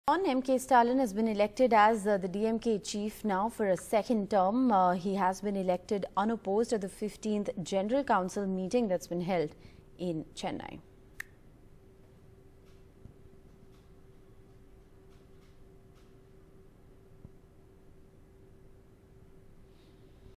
MK Stalin Elected Unopposed As DMK Chief For 2nd Time
DMK stalwart and Tamil Nadu Chief Minister M K Stalin was unanimously elected as party president at the party's general council meeting held in Chennai on Sunday.